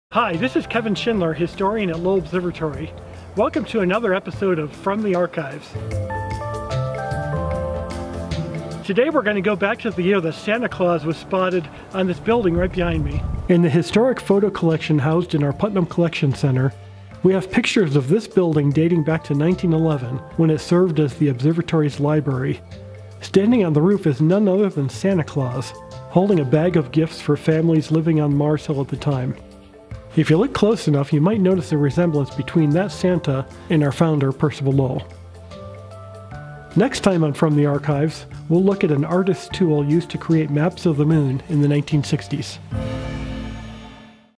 A Santa Spotting on Mars Hill
Many years ago, Santa was spotted up here on Mars Hill and we actually have pictorial proof in our archives.